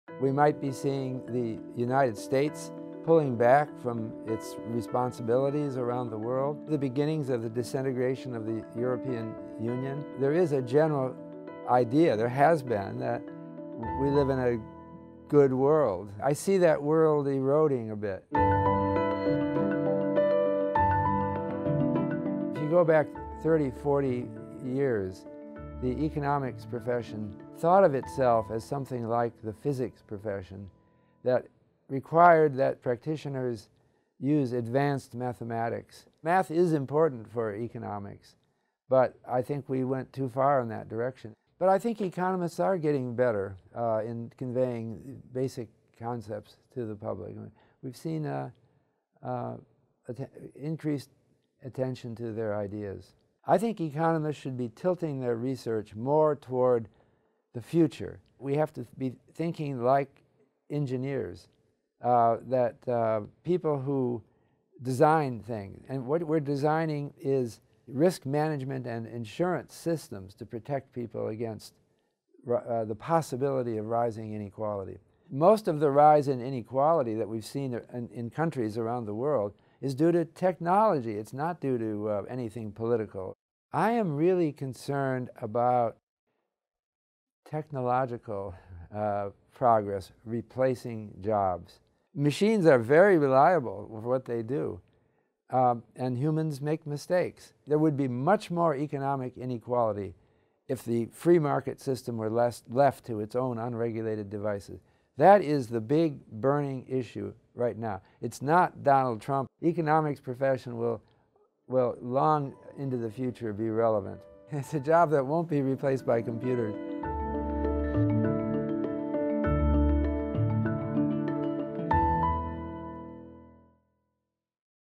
Nobel Laureate Robert Shiller on the Future of Economics
Nobel Laureate Prof. Robert J. Shiller was awarded the Sveriges Riksbank Prize in Economic Sciences in Memory of Alfred Nobel in 2013 together with Eugene F. Fama and Lars Peter Hansen 'for their empirical analysis of asset prices.'

In this video, he takes a look at the future of economics and suggests that economists should work on designing systems that protect people against the possibility of rising inequality.